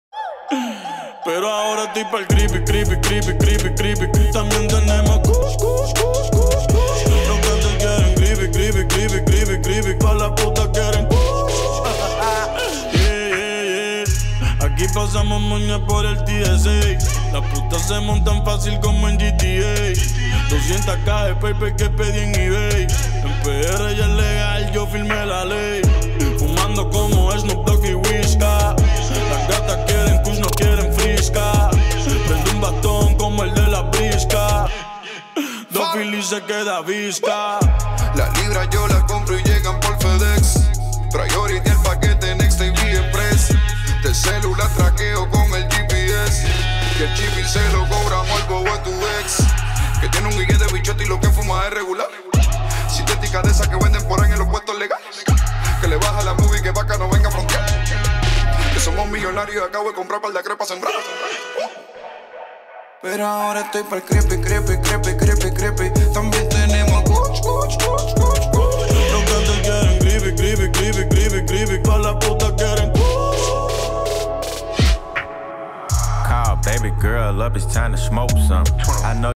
Telescopic Crane with 360 head (3 axis)
Music Video with our Telescopic Crane & 3 axis 360 remote head. Farruko, Bad Bunny, Nicky Minaj & Travis Scott.